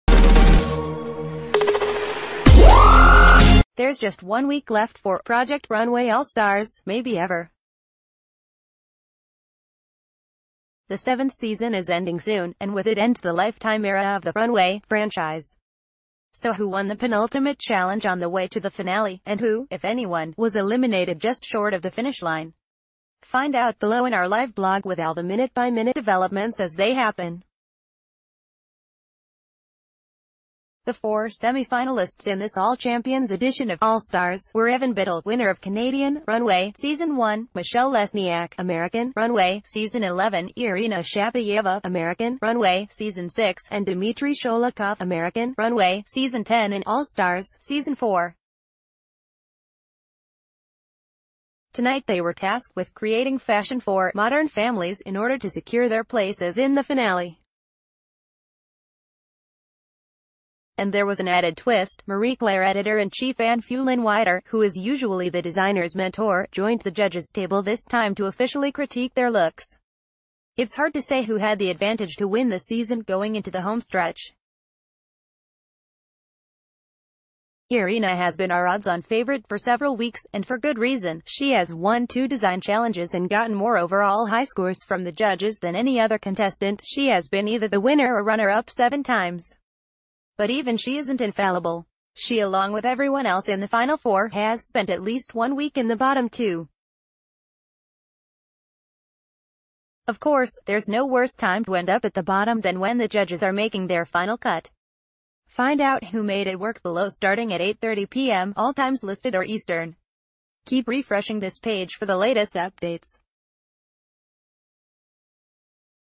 'Project Runway All Stars' recap: Who advanced to the finale after Anne Fulenwider judged 'Modern...
'Project Runway All Stars' recap: Who advanced to the finale after Anne Fulenwider judged 'Modern Families'? [UPDATING LIVE BLOG]
There’s just one week left for “Project Runway All Stars” — maybe ever. The seventh season is ending soon, and with it ends the Lifetime era of the “Runway” franchise. So who won the penultimate challenge on the way to the finale, and who (if anyone) was eliminated just short of the finish line? Fin...